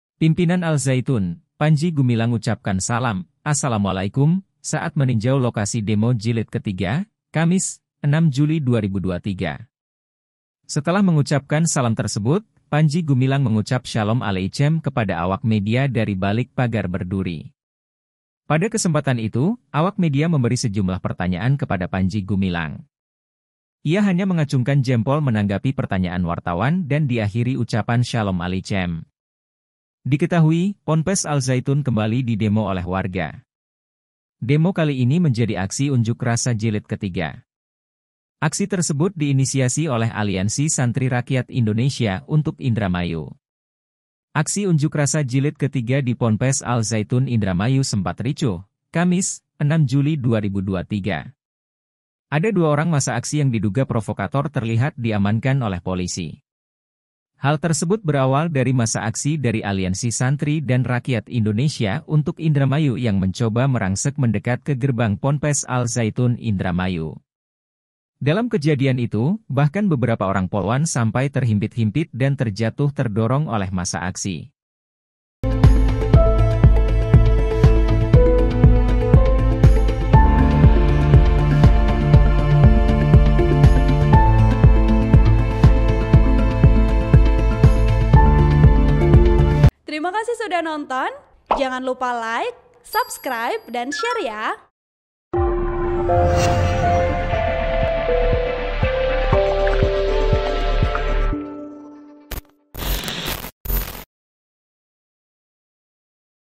Massa Demo 'Jilid III' Sempat Ricuh, Panji Gumilang Ucapkan Shalom Aleichem & Acungkan Jempol
VP : ARIFAH
TRIBUNMATARAMAN.COM, INDRAMAYU - Pimpinan Al Zaytun, Panji Gumilang ucapkan salam 'Assalamualaikum' saat meninjau lokasi demo jilid III hari ini, Kamis (6/7/2023).

Setelah mengucapkan salam tersebut, Panji Gumilang mengucap Shalom Aleichem kepada awak media dari balik pagar berduri.

Pada kesempatan itu, awak media memberi sejumlah pertanyaan kepada Panji Gumilang.

Artikel ini telah tayang di TribunCirebon.com dengan judul Panji Gumilang Tinjau Lokasi Demo Jilid III, Ucapkan Shalom Aleichem dan Acungkan Jempol,
Penulis: Handhika Rahman | Editor: Mutiara Suci Erlanti